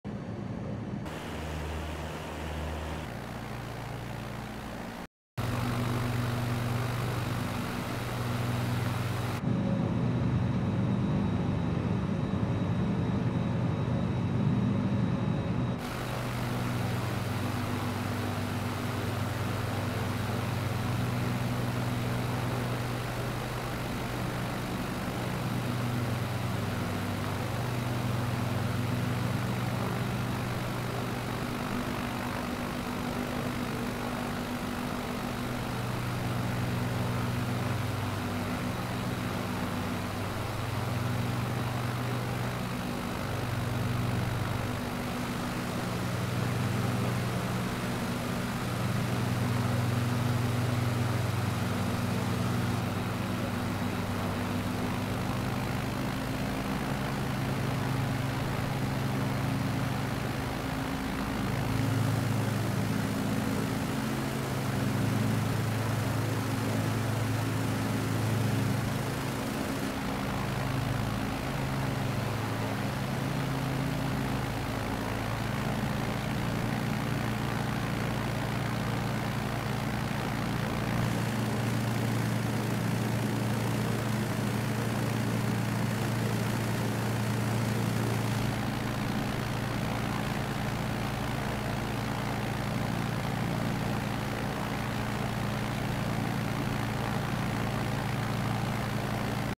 【HD】ダムフライト～ 宮崎県 / 広渡ダム・いたや湖 （日本のダム湖 No.500 最終）
今回は、宮崎県日南市北郷町北河内に造られた
広渡（ひろと）ダム（いたや湖）です。
使用機種：Zlin Aircraft Shock Ultra